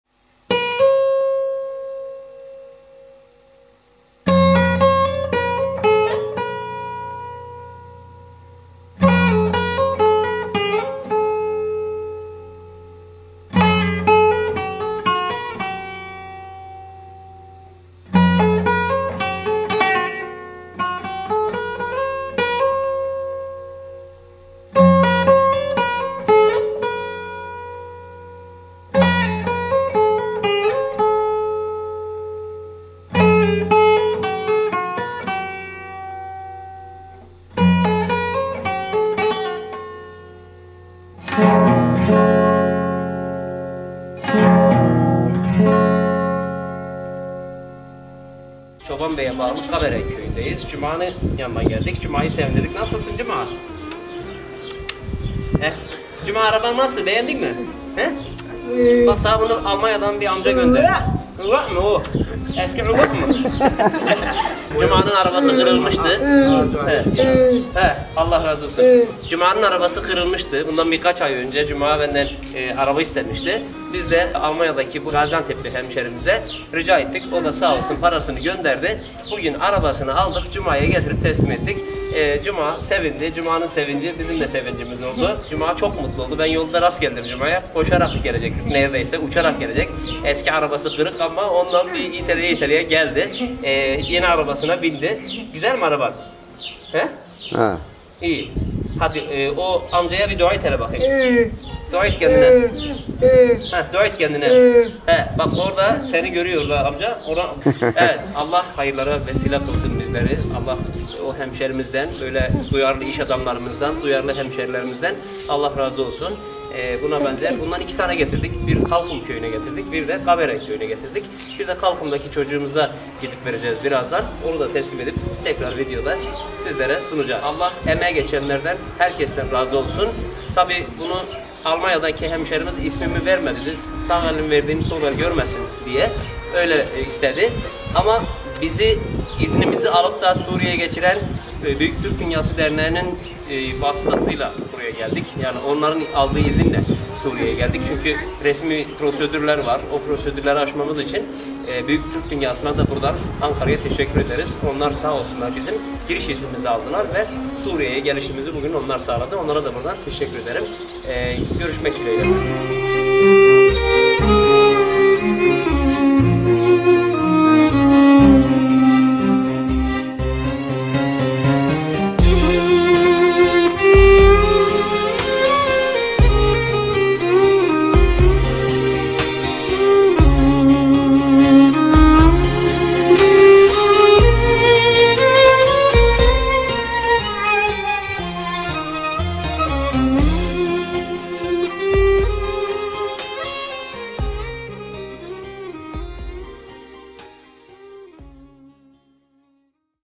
Cuma ve İsmail'in sandalye sevinci
Engelli Cuma ve İsmail kardeşlerimizin sandalyelerini ilettik, Sandalyeleri Almanya da bulunan Gaziantep'li iş adamımız Recep Yılmaz hediye etti.

Merhaba Gönül dostlarım; Ben Adil ŞAN.
Suriye Türkmenleri ile alakalı tüm videoları kanalımdan takip edebilirsiniz.